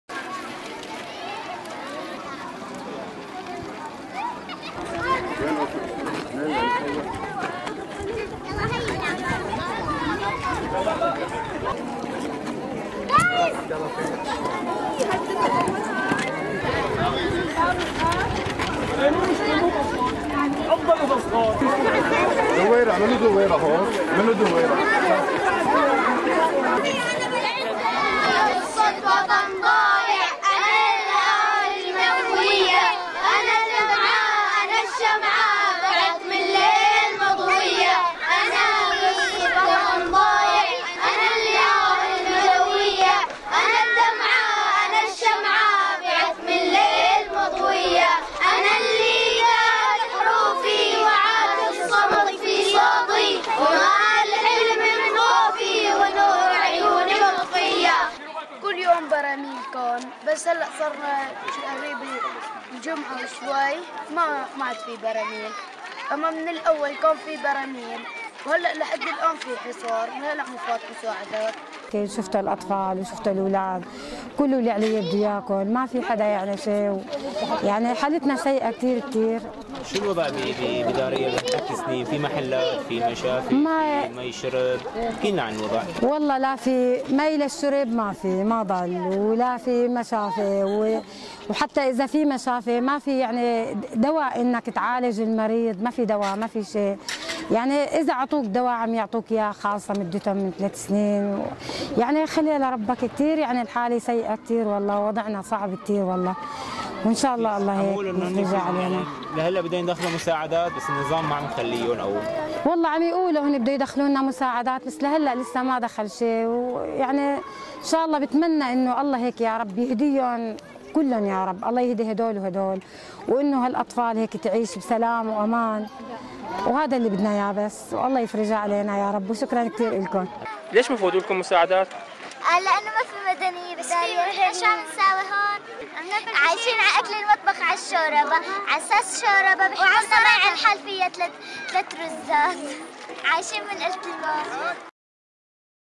Daraya - break the siege protest
Women and children protest in Daraya over the blockade.